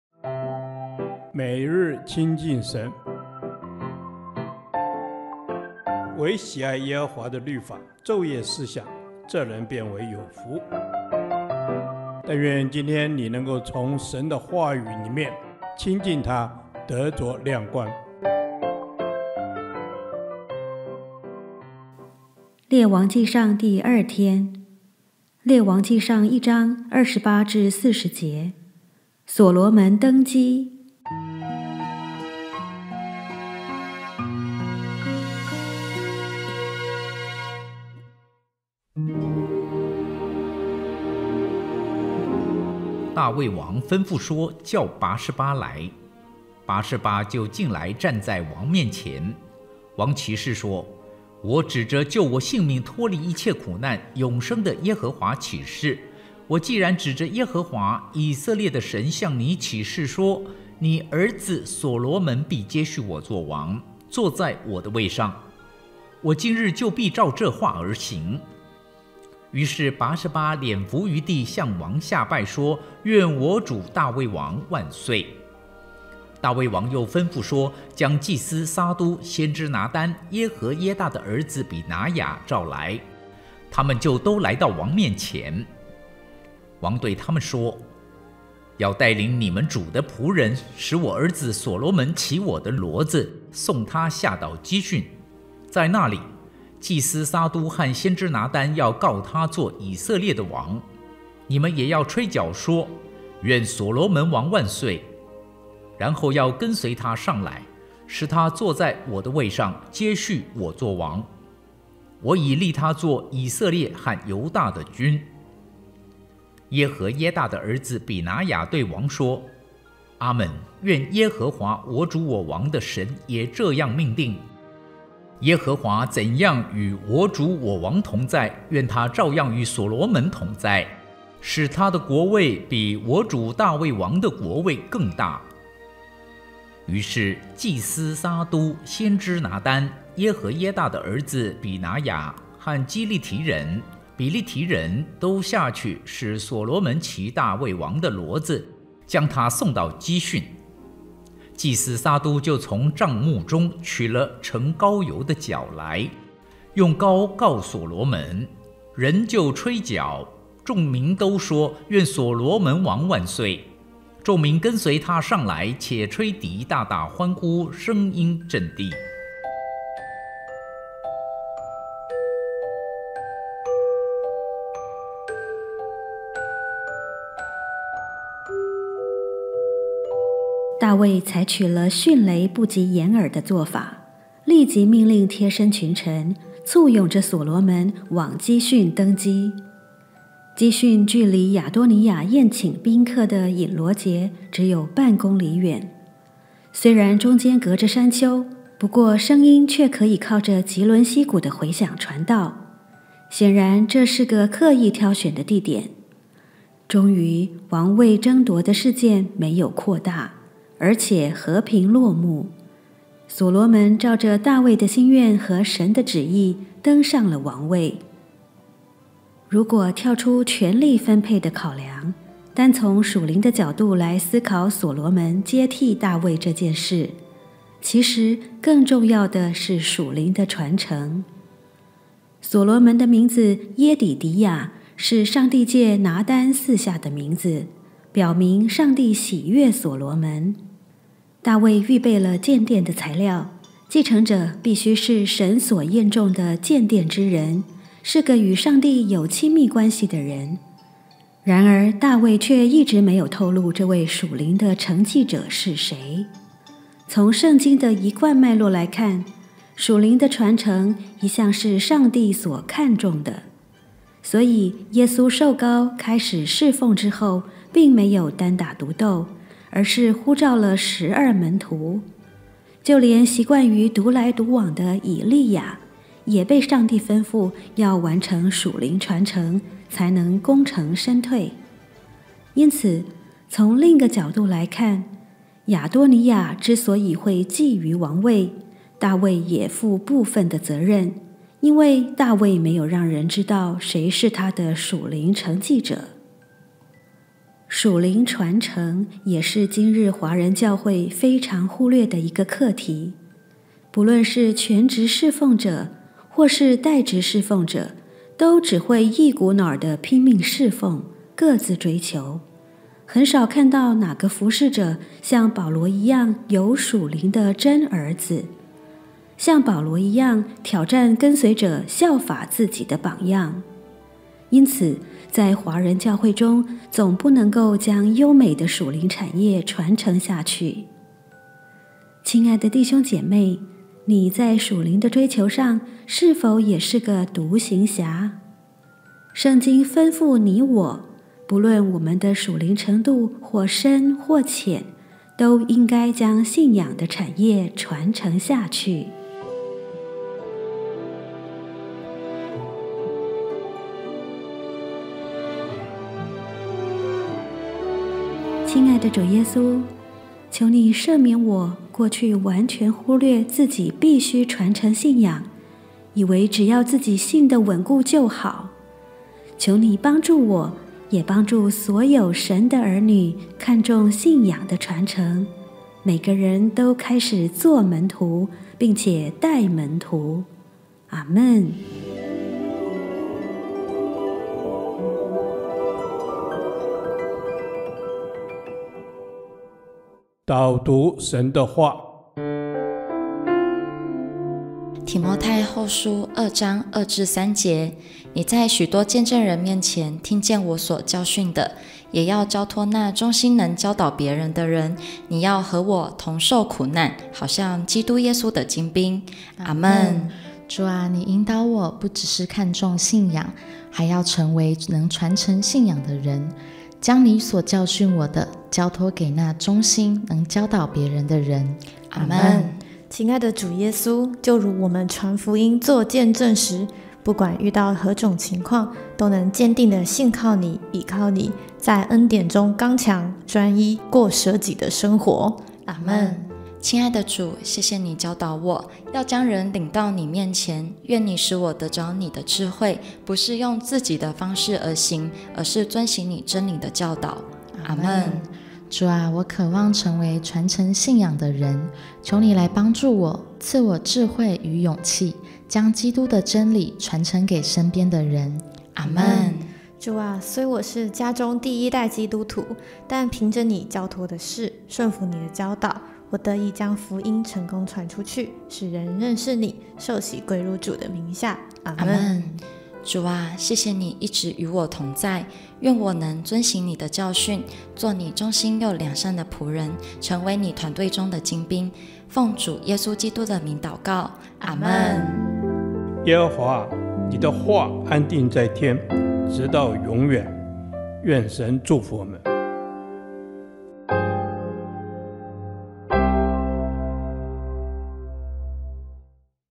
20240521 所羅門登基 [每日親近神-列王紀上]
經文：列王紀上一章28~40節
主題：所羅門登基

大衛採取了迅雷不及掩耳的作法，立即命令貼身群臣，簇擁著所羅門往基訓登基；基訓距離亞多尼雅宴請賓客的隱羅結只有半公里遠，雖然中間隔著山丘，不過聲音卻可以靠著汲淪溪谷的迴響傳到，顯然這是個刻意挑選的地點。終於，王位爭奪的事件沒有擴大，而且和平落幕；所羅門照著大衛的心願和神的旨意，登上了王位。

如果跳出權力分配的考量，單從屬靈的角度來思考所羅門接替大衛這件事，其實更重要的是屬靈的傳承。所羅門的名字「耶底底亞」是上帝藉拿單賜下的名字，表明上帝喜悅所羅門（參撒下十二24~25）；大衛預備了建殿的材料，繼承者必須是神所驗中的建殿之人，是個與上帝有親密關係的人。然而，大衛卻一直沒有透露這位屬靈的承繼者是誰。從聖經的一貫脈絡來看，屬靈的傳承一向是上帝所看重的，所以耶穌受膏開始事奉之後，並沒有單打獨鬥，而是呼召了十二門徒；就連習慣於獨來獨往的以利亞，也被上帝吩咐要完成屬靈傳承，才能功成身退。因此，從另個角度來看，亞多尼雅之所以會覬覦王位，大衛也負部分的責任，因為大衛沒有讓人知道誰是他的屬靈承繼者。

「屬靈傳承」也是今日華人教會非常忽略的一個課題，不論是全職事奉者，或是帶職事奉者，都只會一股腦兒的拼命事奉，各自追求。很少看到哪個服事者，像保羅一樣有「屬靈的真兒子」，像保羅一樣挑戰跟隨者「效法自己的榜樣」。因此在華人教會中，總不能夠將優美的屬靈產業傳承下去。親愛的弟兄姊妹，你在屬靈的追求上，是否也是個獨行俠？聖經吩咐你我，不論我們的屬靈程度或深或淺，都應該將信仰的產業傳承下去。

回應：親愛的主耶穌！求祢赦免我過去完全忽略自己必須傳承信仰，以為只要自己信得穩固就好；求祢幫助我，也幫助所有神的兒女看重信仰的傳承，每個人都開始作門徒，並且帶門徒。阿們！

禱讀：提摩太後書二章2~3節
你在許多見證人面前聽見我所教訓的，也要交託那忠心能教導別人的人。你要和我同受苦難，好像基督耶穌的精兵。
☀️ 同步發行【每日親近神 】靈修手冊，歡迎線上訂購*網址

☀️影像及音樂、圖片、文字版權均屬天聲傳播協會所有***